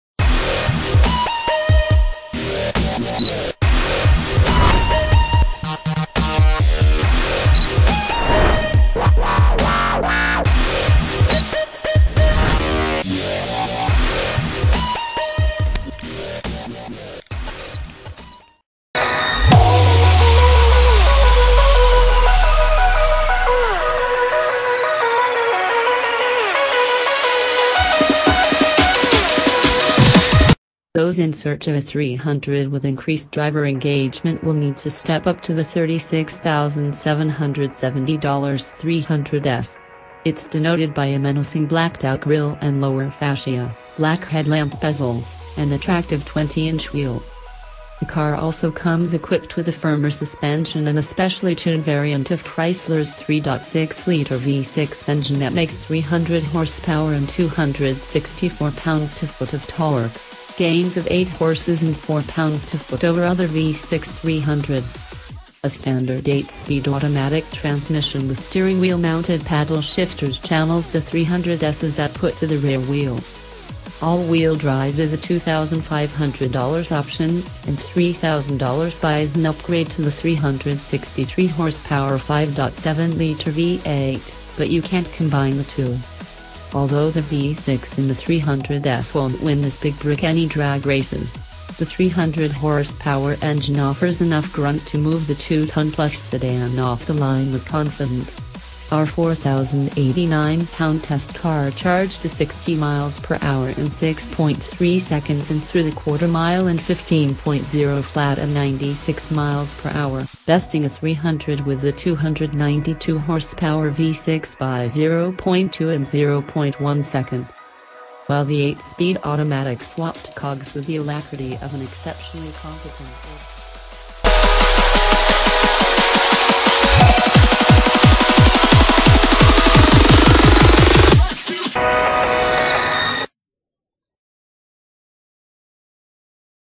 Watch Now! 2018 Chrysler 300 Prices
Watch Now! 2018 Chrysler 300 Prices - Those in search of a 300 with increased driver engagement will need to step up to the $36,770 300S. It’s denoted by a menacing blacked-out grille and lower fascia, black headlamp bezels, and attractive 20-inch wheels. The car also comes equipped with a firmer suspension and a specially tuned variant of Chrysler’s 3.6-liter V-6 engine that makes 300 horsepower and 264 lb-ft of torque (gains of 8 horses and 4 lb-ft over other V-6 300s). A standard eight-speed automatic transmission with steering-wheel-mounted paddle shifters channels the 300S’s output to the rear wheels. All-wheel drive is a $2500 option, and $3000 buys an upgrade to the 363-hp 5.7-liter V-8—but you can’t combine the two.

‎$33,435

Popular Post !